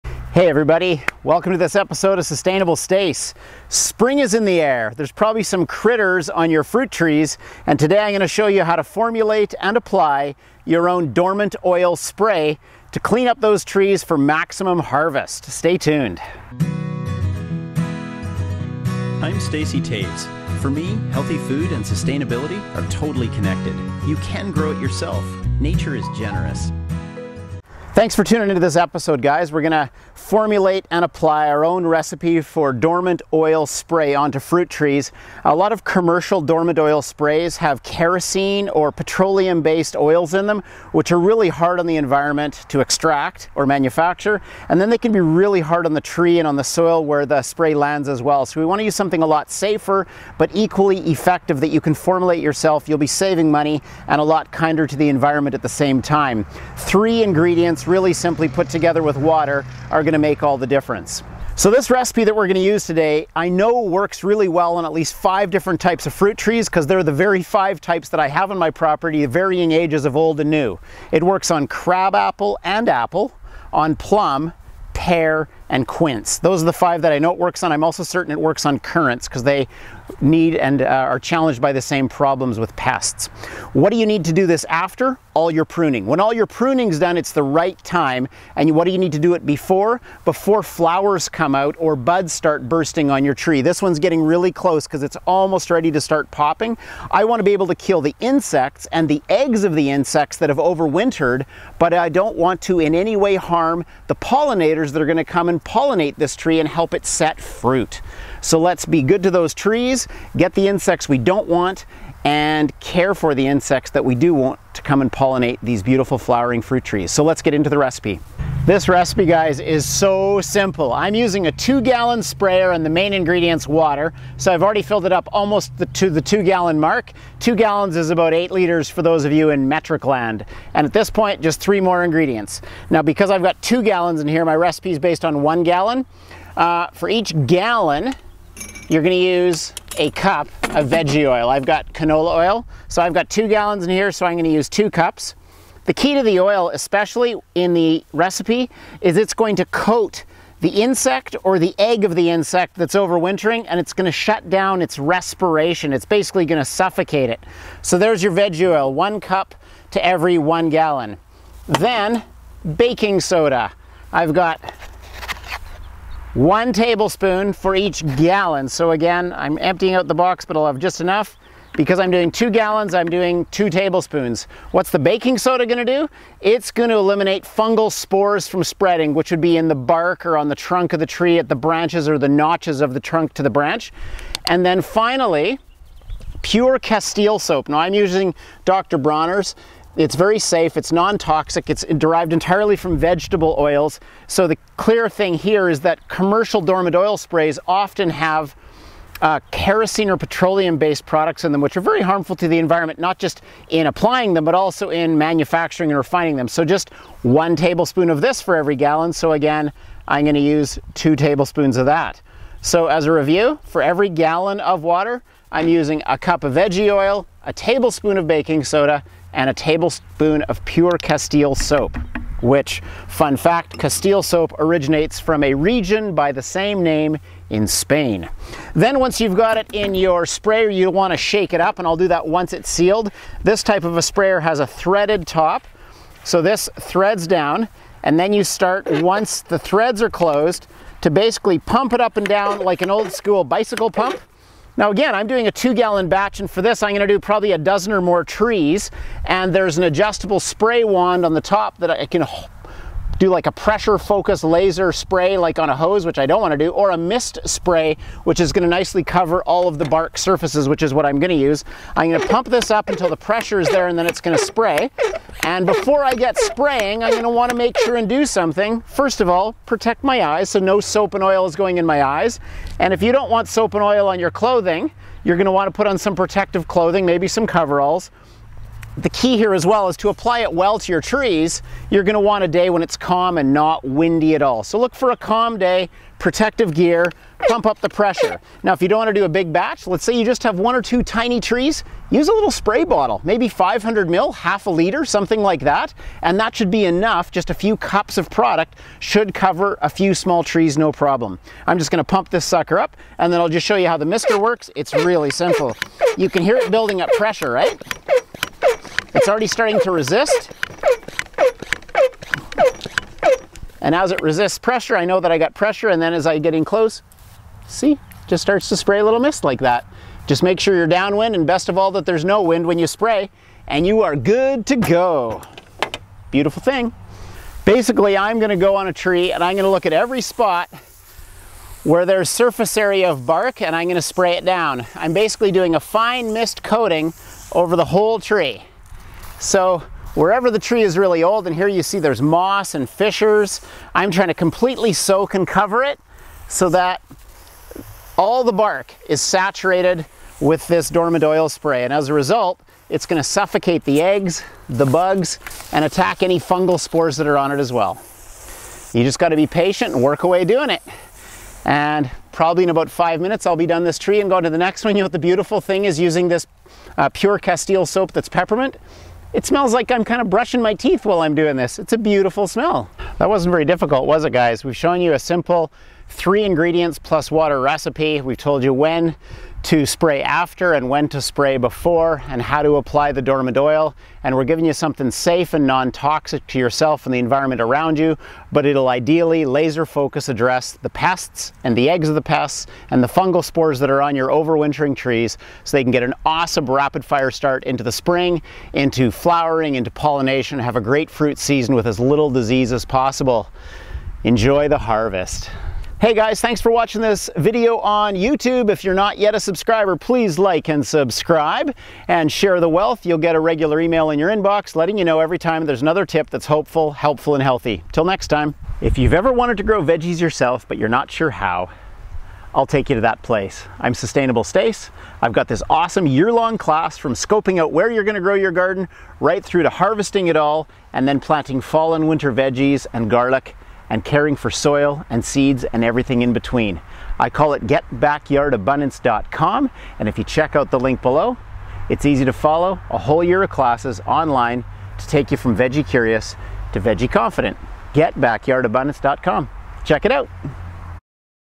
How To Make DIY Organic Pest Spray For Your Fruit Trees!
In this video I will explain how to formulate and apply your own dormant oil spray for fruit trees.

The goal, with properly applied dormant oil spray, is that the fruit tree is rendered free of harmful bugs as well as their eggs and fungal spores.

This recipe has been proven for me on the following fruit trees: crabapple, apple, plum, pear, quince & currants.

There are optimal time windows to apply the spray, I’ll explain what needs to happen BEFORE and AFTER the application so that you can be sure your timing is right on!

For each gallon (~4 liters) of water you add these 3 ingredients:
1 cup veg oil
1 tablespoon baking soda
1 tablespoon pure castile soap